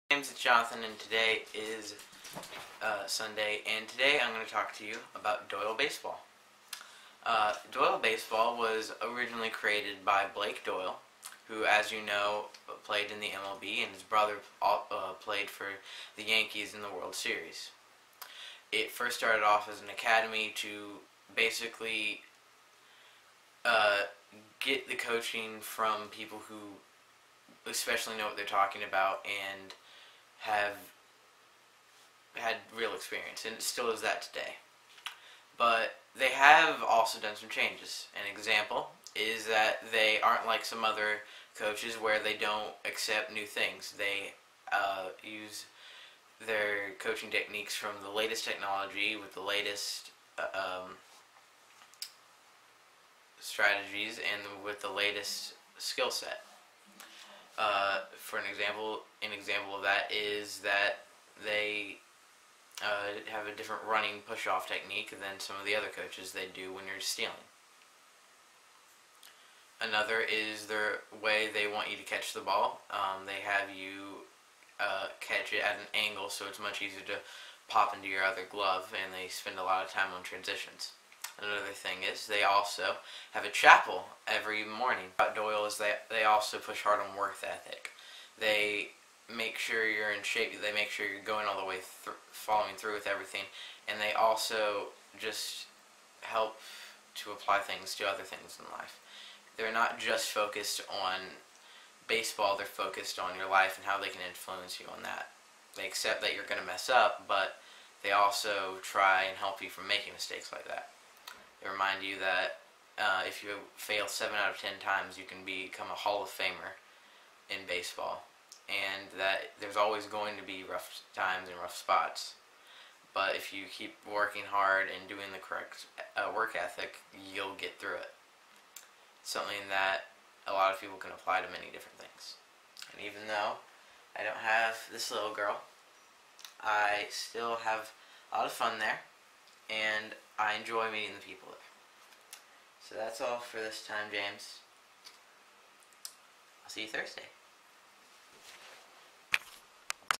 Doyle Baseball Achademy
A video in which Jonathan Talks about Doyle Baseball Academy